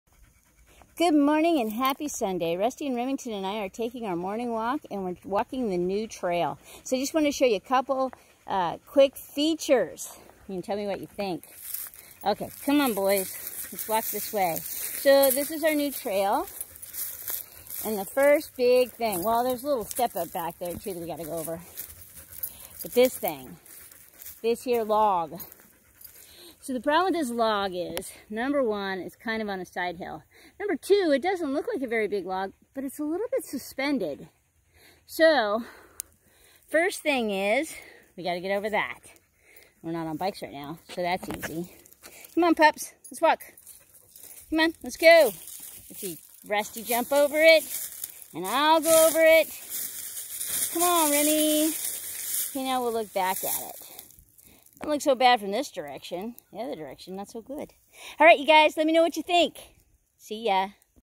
The new trail/log on a sidehill
Walking the new trail with Rusty and Remington.